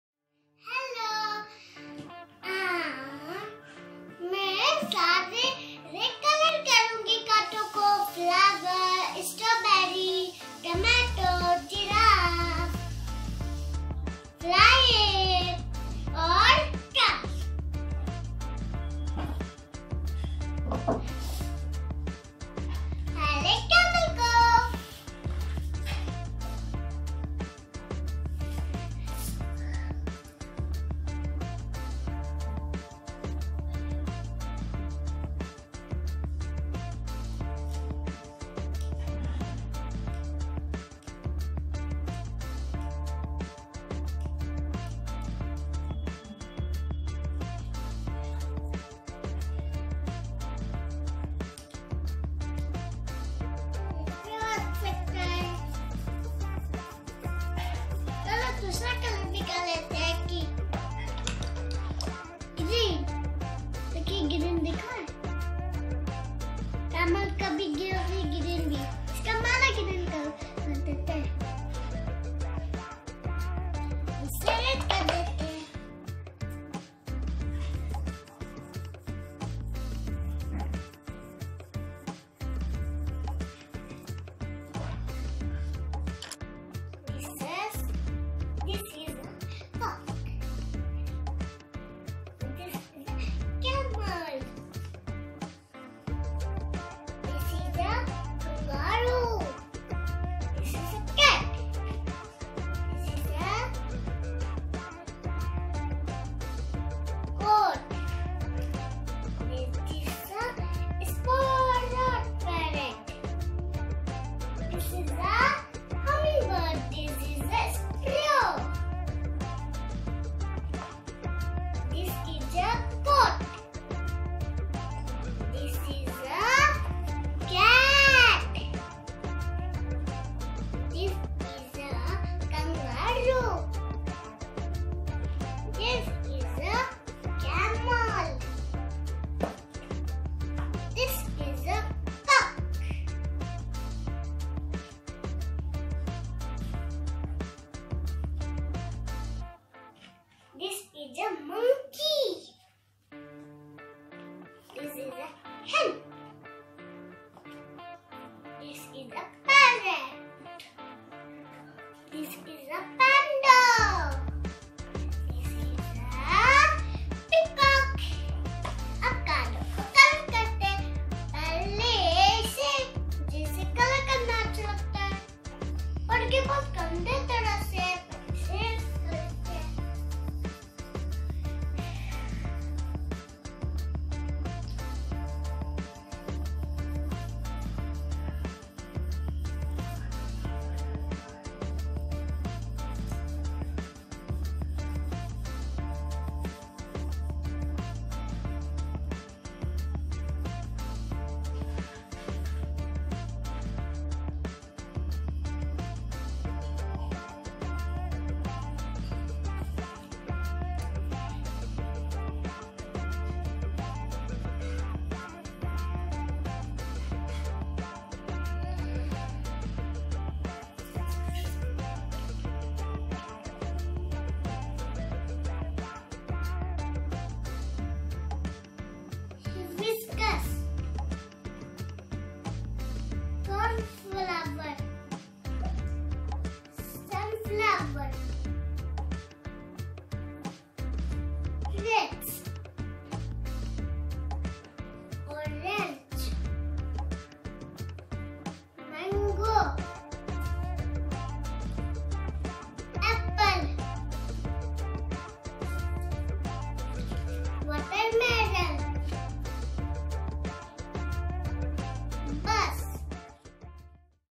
Colour and wipe, pinki idea, कार्ड में रंग भरें, बच्चों को खिलौने से सिखाये
Colour and wipe, pinki idea, जानवर, फल, फूल, को पहचान करना सिखाये